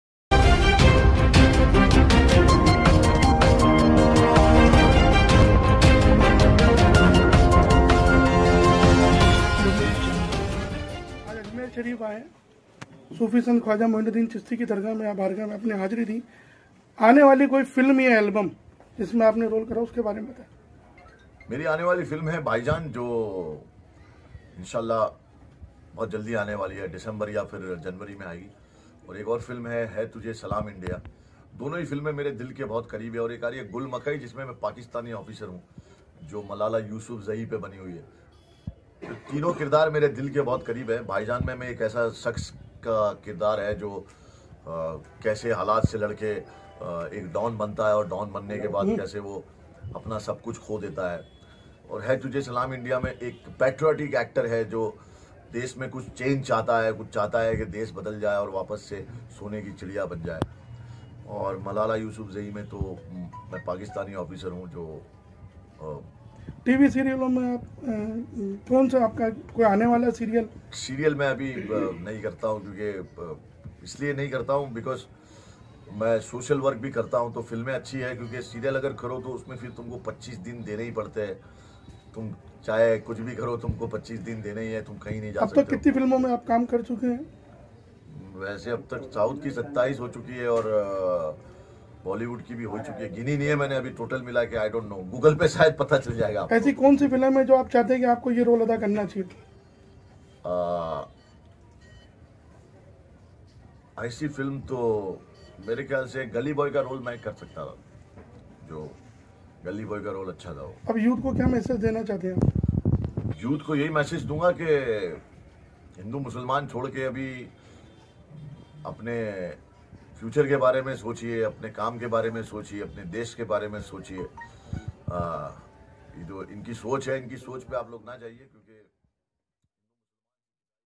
AYN NEWS फिल्म अभिनेता एजाज खान ने की दरगाह ख्वाजा मोइनुद्दीन चिश्ती की जियारत
फिल्म अभिनेता एजाज खान ने की दरगाह ख्वाजा मोइनुद्दीन चिश्ती की जियारत।
एजाज खान हमेशा विवादों में रहे उन्होंने हमेशा कोई ना कोई मुद्दा रखा
 एजाज खान ने अपने पूरे परिवार के साथ ख्वाजा मोइनुद्दीन चिश्ती की चौखट चुम्मी व अकीदत के फूल पेश किए।

The film actor Ejaz Khan performed the dargah of Khwaja Moinuddin Chishti.
 Ijaz Khan was always in controversy, he always kept some issue.
  Ejaz Khan presented Khwaja Moinuddin Chishti's frame, kisses and akidat flowers with his entire family